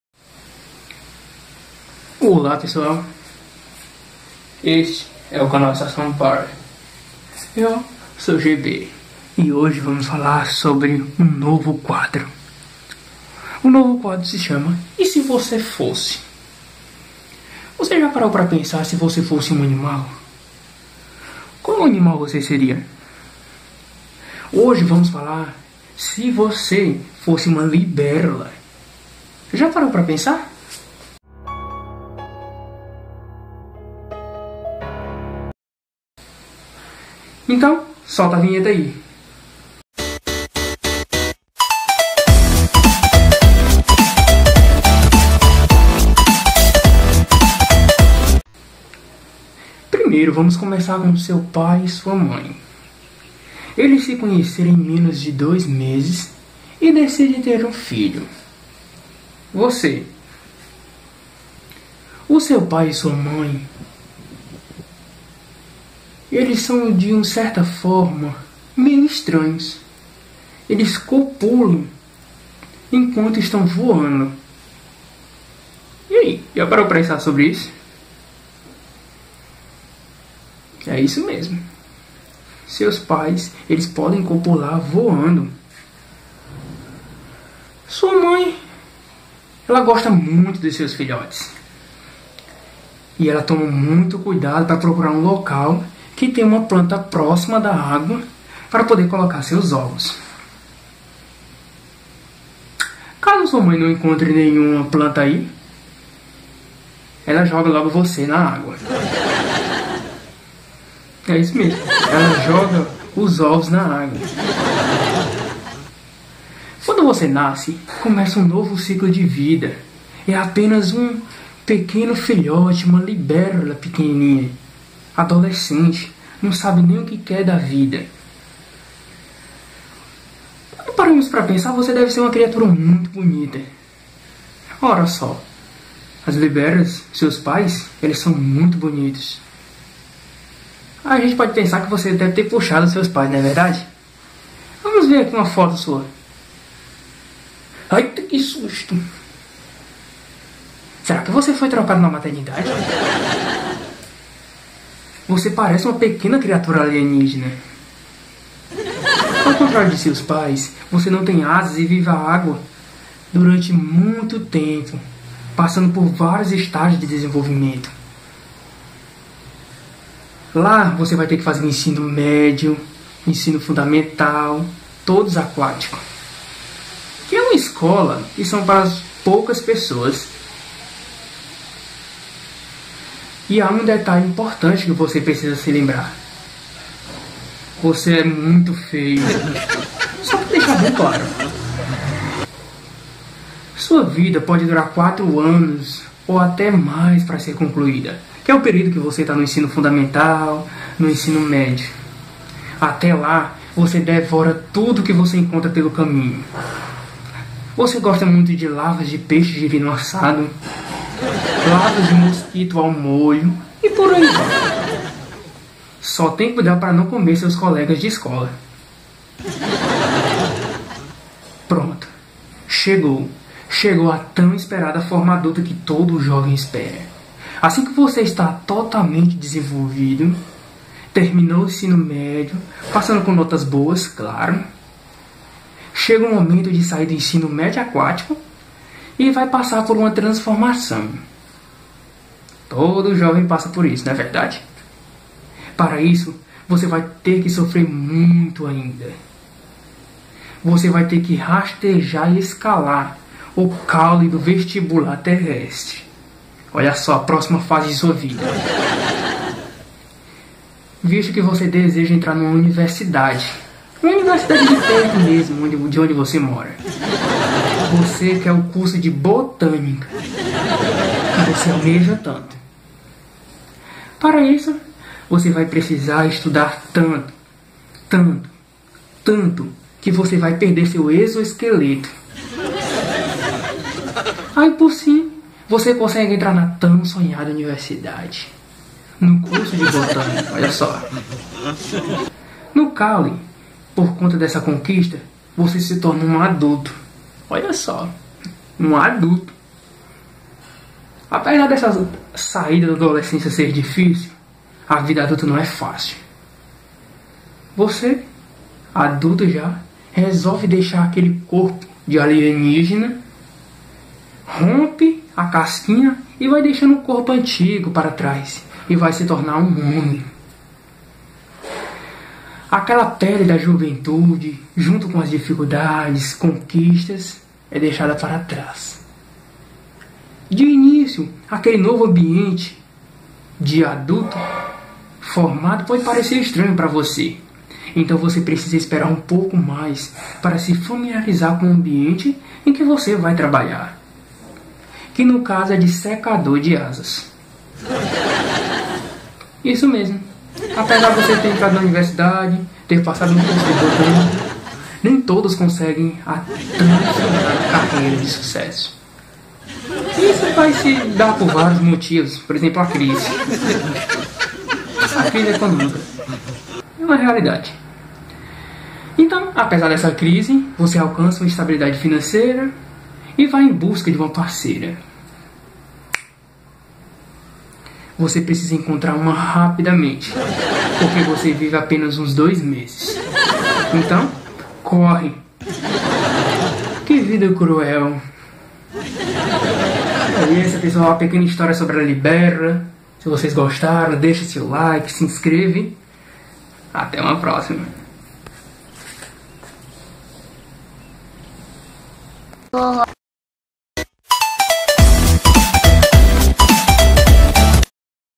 E SE VOCÊ FOSSE UMA LIBÉLULA?
Você já parou para pensar se você fosse um animal? Qual animal você queria ser? Hoje vamos falar se você fosse uma libélula? Primeiro vamos começar com seu pai e sua mãe.